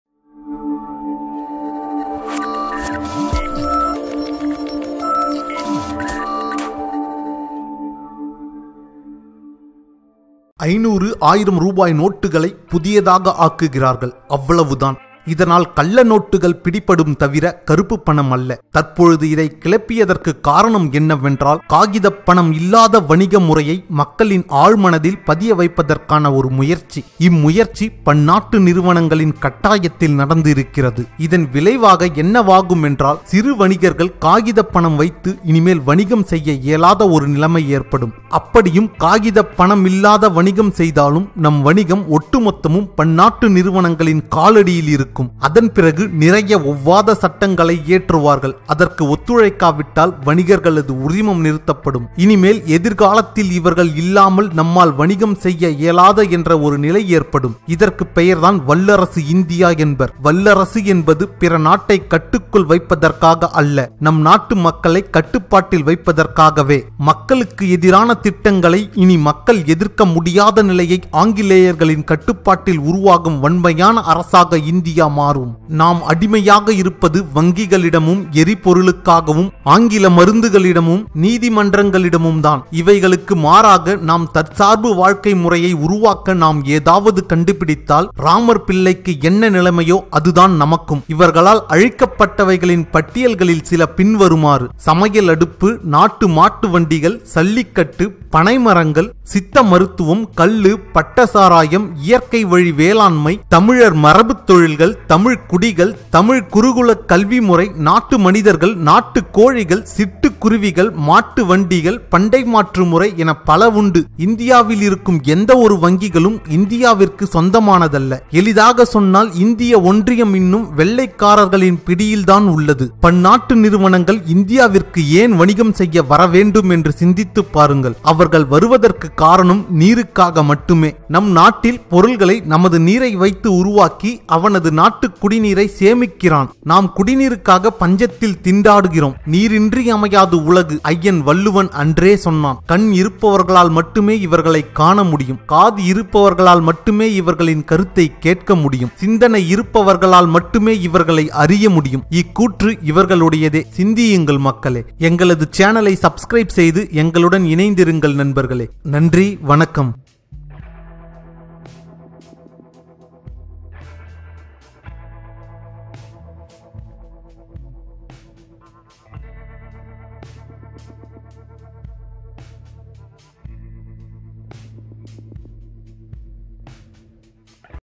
இது கருப்பு பணத்தை உண்மையில் வெளி கொண்டுவருமா ? | BioScope
மத்திய அரசின் இந்த யுக்தி உண்மையில் கருப்பு பணத்தை வெளி கொணருமா ?

நன்றி சுரேசு இலட்சுமணன்